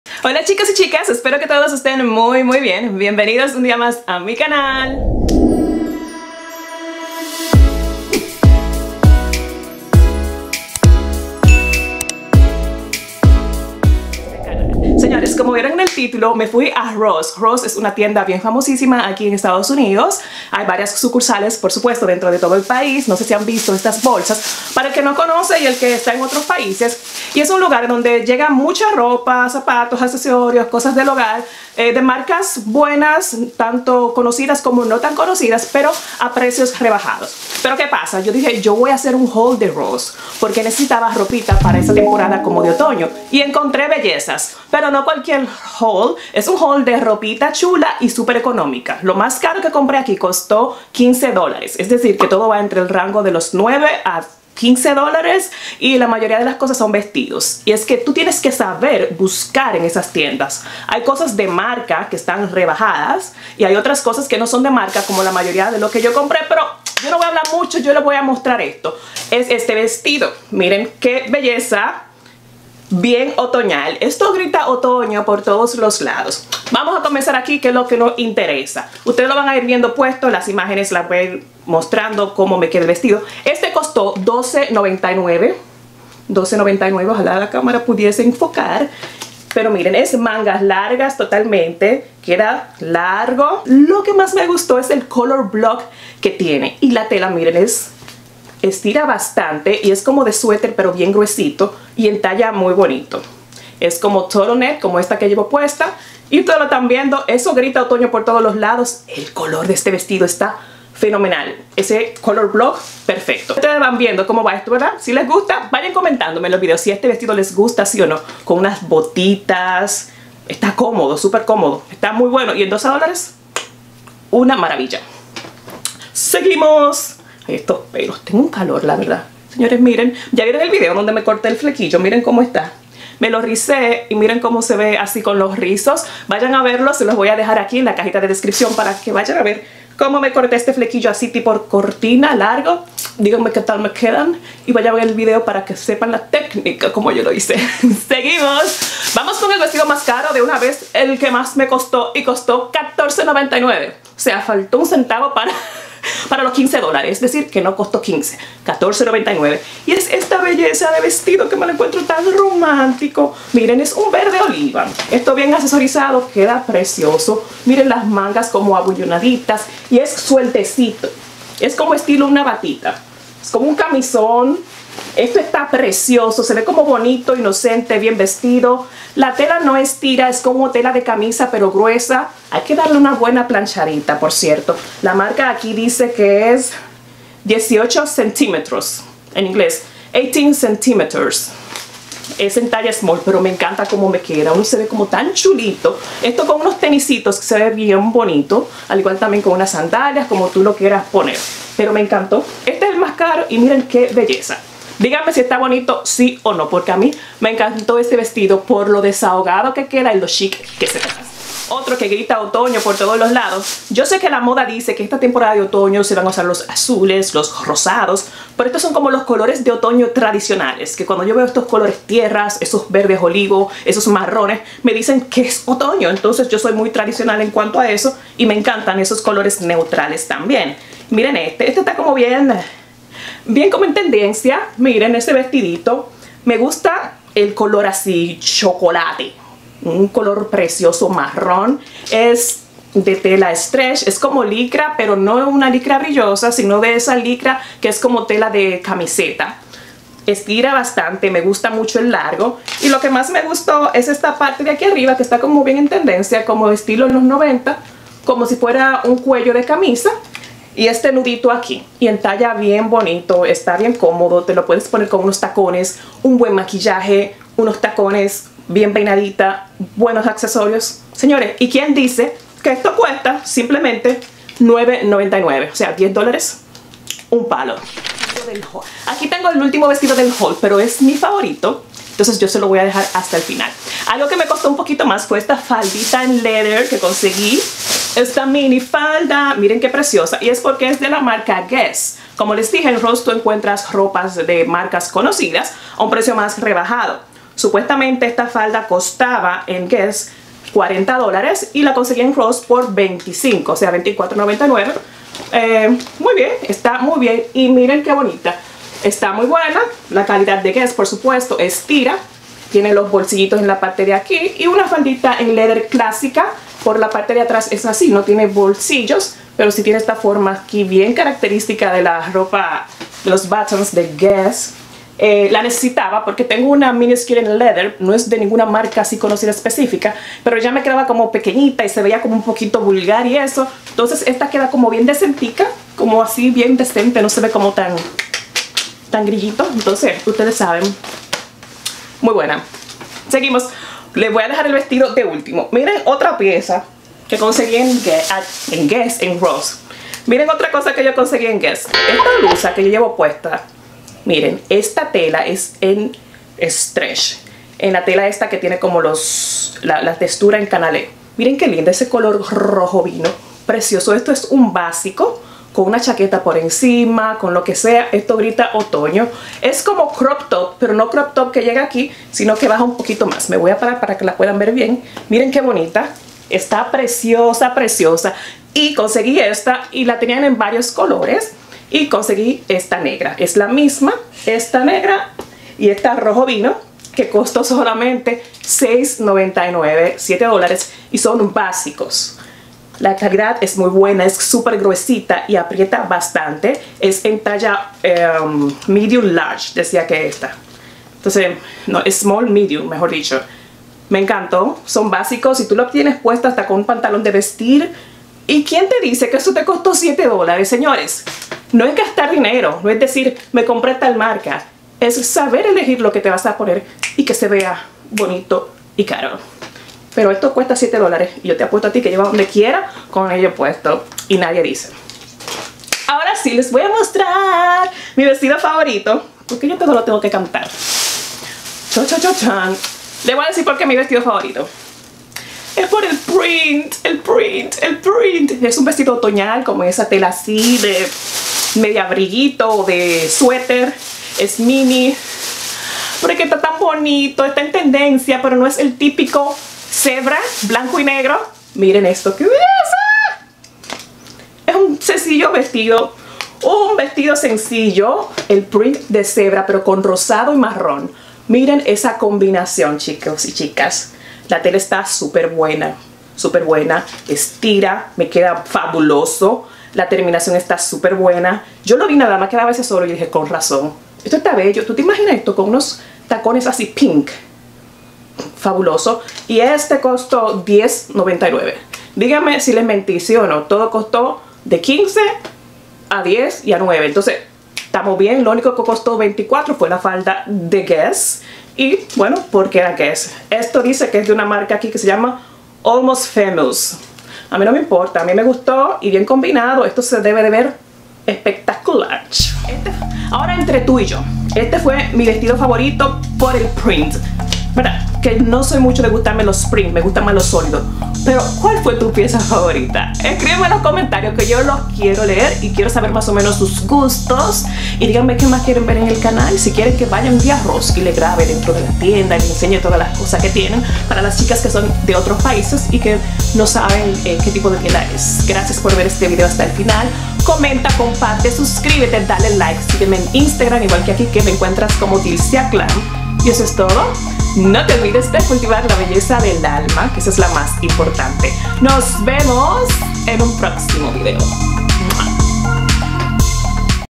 HAUL DE ROSS OTOÑO 2022 , ROPA BARATA QUE PARECE CARA | MI SECRETO!! | DILSIAGLAM
HOY LES COMPARTO LAS PRENDAS DE OTOÑO QUE ENCONTRE EN ROSS STORES ,PIEZAS ESENCIALES A MENOS DE 15.00 DOLORES, ESPERO Y LES GUSTE!!